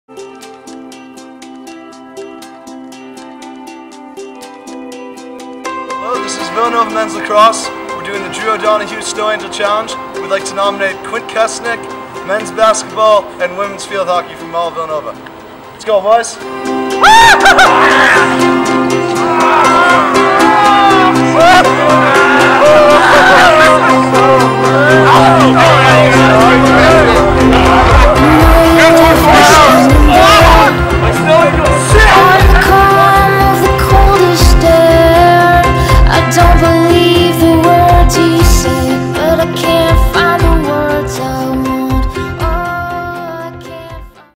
Men's Lacrosse- Drew O'Donoghue Snow Angel Challenge
The Villanova Men's Lacrosse team took part in the Drew O'Donoghue Snow Angel Challenge, in preparation for their Drew Fund game on April 18th vs. St. John's. The Drew Fund is a foundation created in honor of the late Drew O'Donoghue (Villanova class of 2014) who passed away on 2-14-14 after a year long battle with Adrenal Cortical Cancer. Drew was an avid NovaNation fan! Quint Kessenich, Villanova Men's Basketball & Villanova Field Hockey, you're up next!